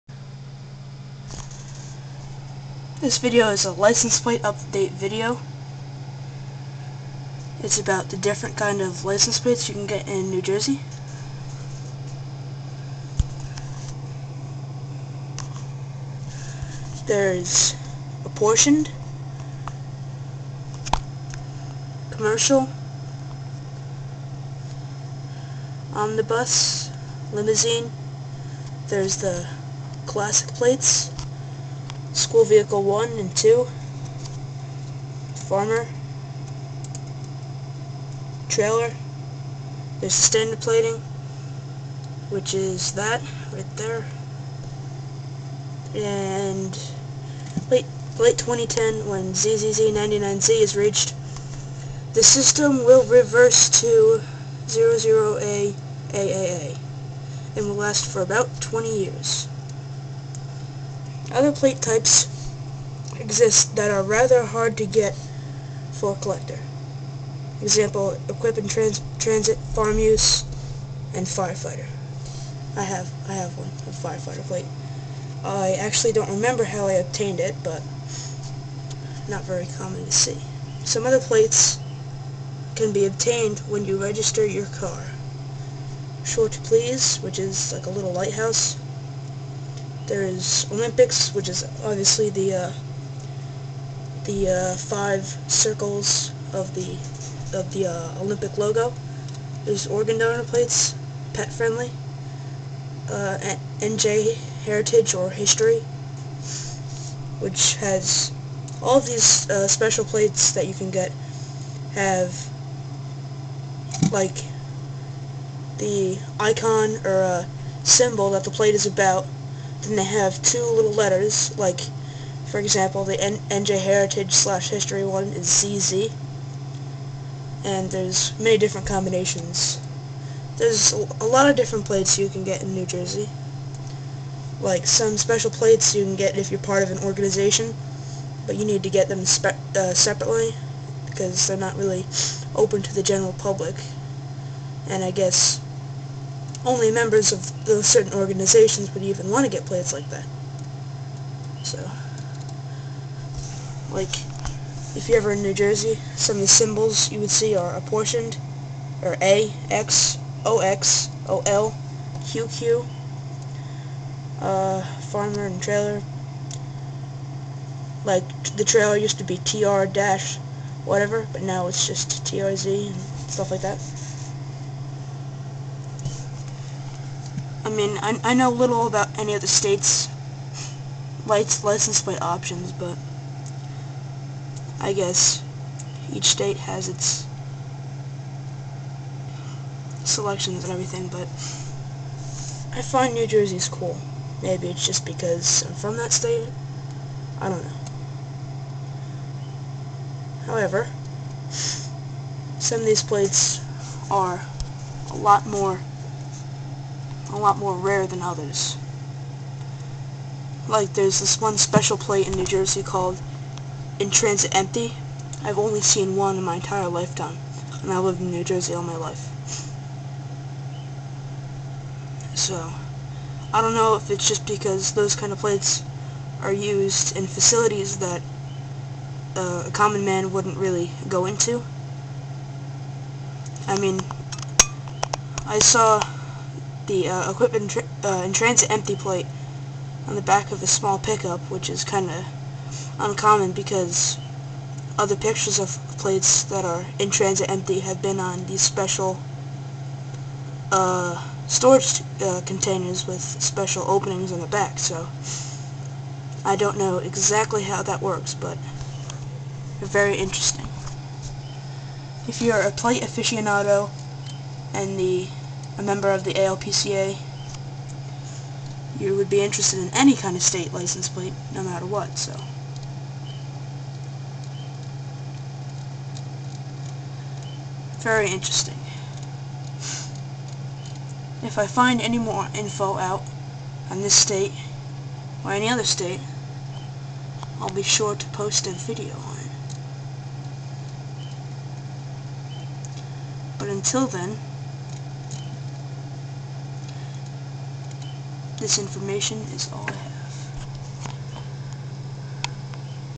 License plate Update
this information is good for a plate collector who is interested in learing about new jersey. i was rished to do this, so the quality might not be 100%.